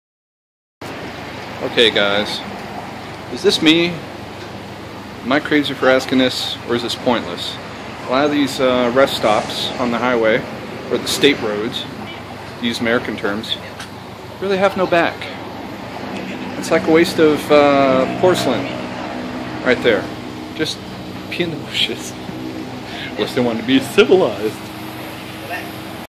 Thailand rest stop toilets
This is one of several videos I made for a visa run a few months ago. I should put everything together in a month.